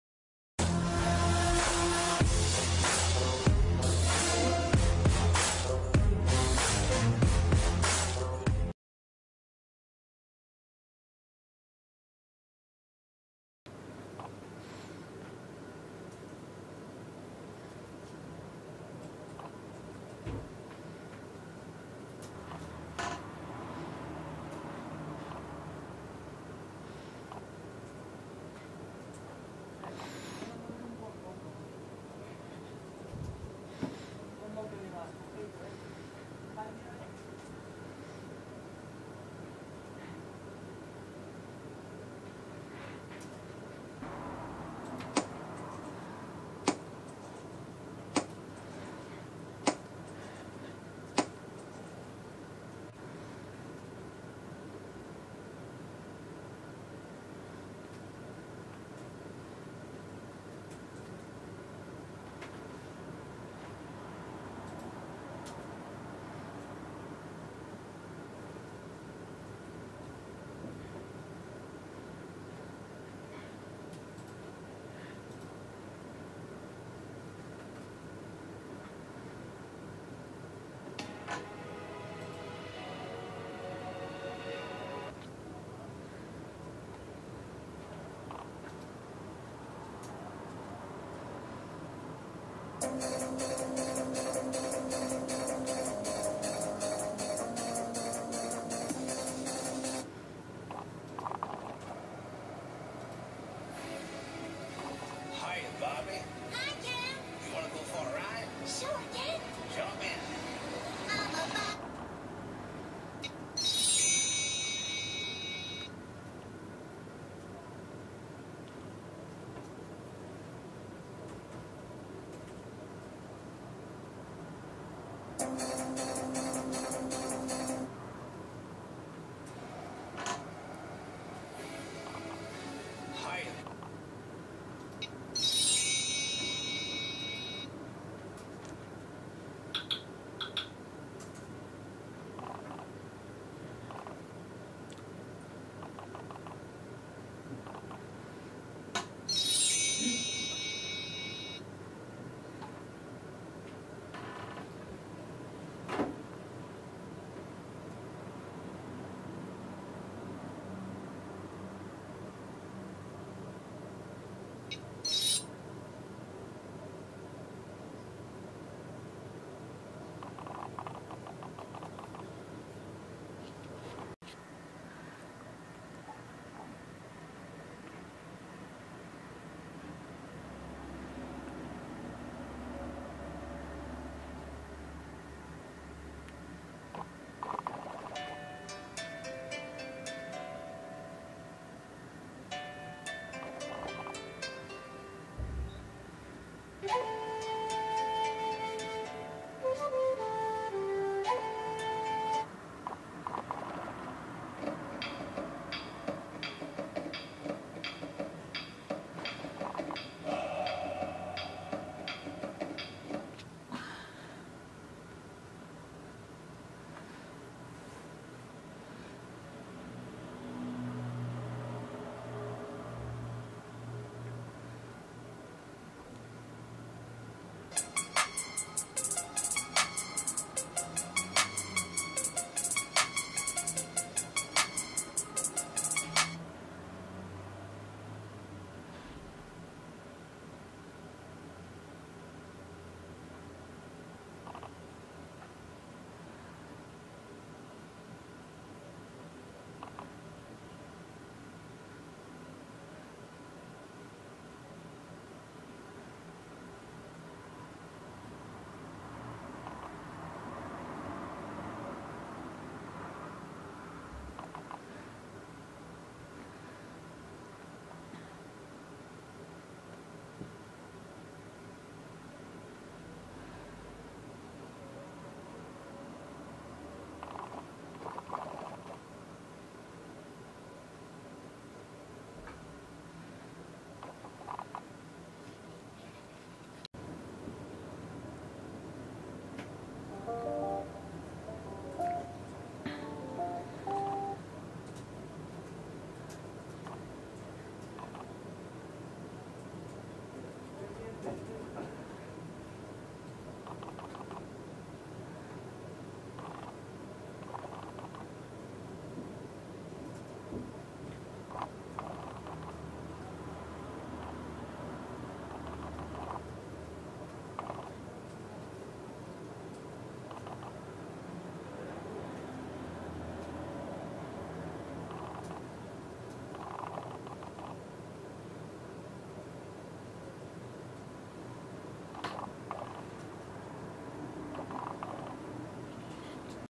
ePub: Ejercicios alumnos 2021
Ejercicios ePub realizados por los alumnos del Grado Superior del ciclo de "Diseño y Edición de Publicaciones impersas y Multimedia" del año 2021.
luisenrike.xyz